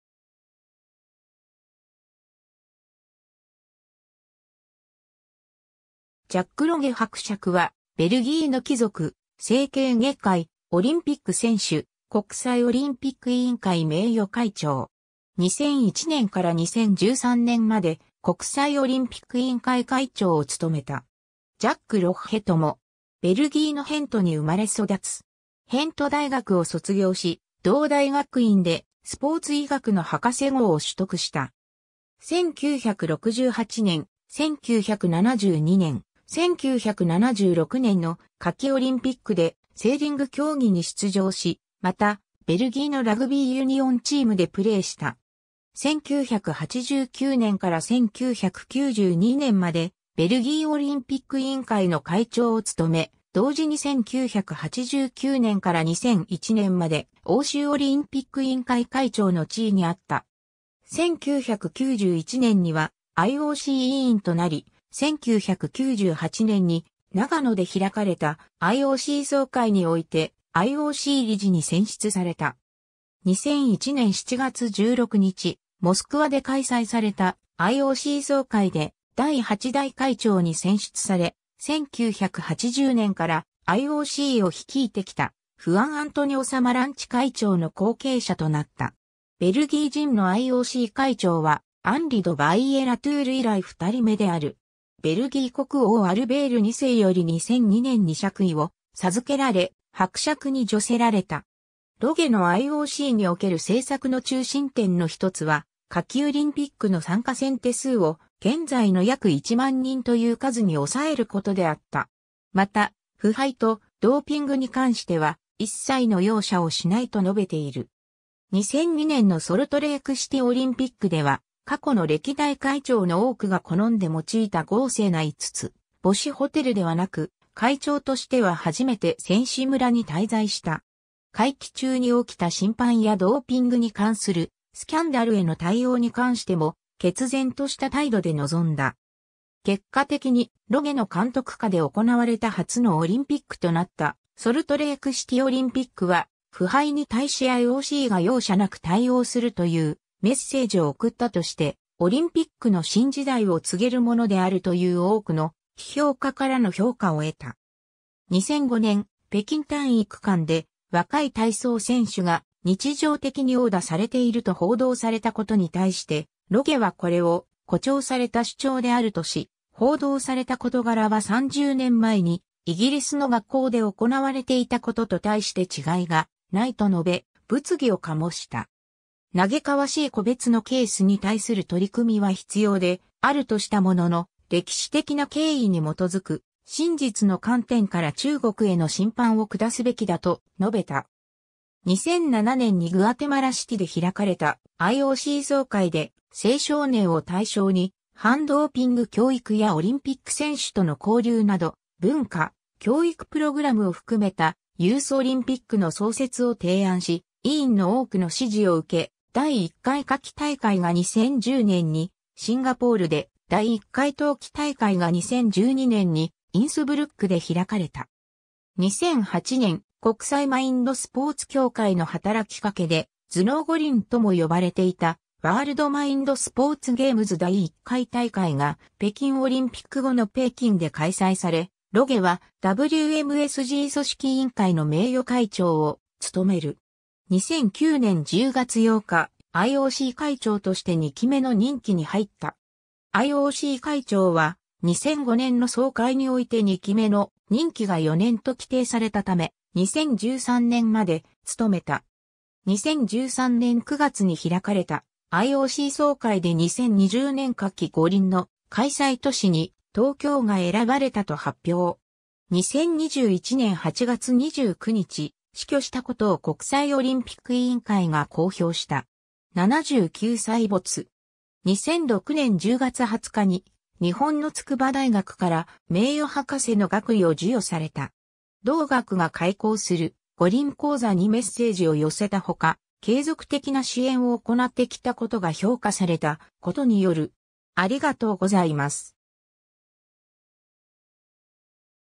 ジャック・ロゲ
ジャック・ロゲ伯爵（Le comte Jacques Rogge, 1942年5月2日 - 2021年8月29日）は、ベルギーの貴族、整形外科医、オリンピック選手、国際オリンピック委員会名誉会長。
2001年から2013年まで国際オリンピック委員会会長を務めた。
ジャック・ロッヘとも。
 ベルギーのヘントに生まれ育つ。
ヘント大学を卒業し、同大学院でスポーツ医学の博士号を取得した。
1968年、1972年、1976年の夏季オリンピックでセーリング競技に出場し、またベルギーのラグビーユニオンチームでプレーした。
1989年から1992年までベルギーオリンピック委員会の会長を務め、同時に1989年から2001年まで欧州オリンピック委員会会長の地位にあった。
1991年にはIOC委員となり、1998年に長野で開かれたIOC総会においてIOC理事に選出された。
 2001年7月16日、モスクワで開催されたIOC総会で第8代会長に選出され、1980年からIOCを率いてきたフアン・アントニオ・サマランチ会長の後継者となった。
ベルギー人のIOC会長はアンリ・ド・バイエ＝ラトゥール以来2人目である。
ベルギー国王アルベール2世より2002年に爵位を授けられ、伯爵に叙せられた。
ロゲのIOCにおける政策の中心点の1つは、夏季オリンピックの参加選手数を現在の約1万人という数に抑えることであった。
また、腐敗とドーピングに関しては一切の容赦をしないと述べている。
 2002年のソルトレイクシティオリンピックでは、過去の歴代会長の多くが好んで用いた豪勢な5つ星ホテルではなく、会長としては初めて選手村に滞在した。
会期中に起きた審判やドーピングに関するスキャンダルへの対応に関しても、決然とした態度で臨んだ。
結果的に、ロゲの監督下で行われた初のオリンピックとなったソルトレイクシティオリンピックは、腐敗に対しIOCが容赦無く対応するというメッセージを送ったとして、オリンピックの新時代を告げるものであるという多くの批評家からの評価を得た。
 2005年、北京体育館で若い体操選手が日常的に殴打されていると報道されたことに対して、ロゲはこれを「誇張された」主張であるとし、「報道された事柄は（中略）30年前にイギリスの学校で行われていたことと大して違いがない」と述べ、物議を醸した。
「嘆かわしい個別のケース」に対する取り組みは必要であるとしたものの、歴史的な経緯に基づく「真実の観点から中国への審判を下すべきだ」と述べた。
2007年にグアテマラシティで開かれたIOC総会で、青少年を対象に反ドーピング教育やオリンピック選手との交流など、文化・教育プログラムを含めたユースオリンピックの創設を提案し、委員の多くの支持を受け、第1回夏季大会が2010年にシンガポールで、第1回冬季大会が2012年にインスブルックで開かれた。
 2008年、国際マインドスポーツ協会（IMSA）の働きかけで「頭脳五輪」とも呼ばれていたワールドマインドスポーツゲームズ（WMSG）第1回大会が北京オリンピック後の北京で開催され、ロゲはWMSG組織委員会の名誉会長を務める。
2009年10月8日、IOC会長として2期目の任期に入った。
IOC会長は、2005年の総会において2期目の任期が4年と規定された為、2013年まで務めた。
2013年9月に開かれたIOC総会で2020年夏季五輪の開催都市に「東京」が選ばれたと発表。
 2021年8月29日、死去したことを国際オリンピック委員会が公表した。
79歳没。
 2006年10月20日に、日本の筑波大学から名誉博士の学位を授与された。
同学が開講する「五輪講座」にメッセージを寄せたほか、継続的な支援を行ってきたことが評価されたことによる。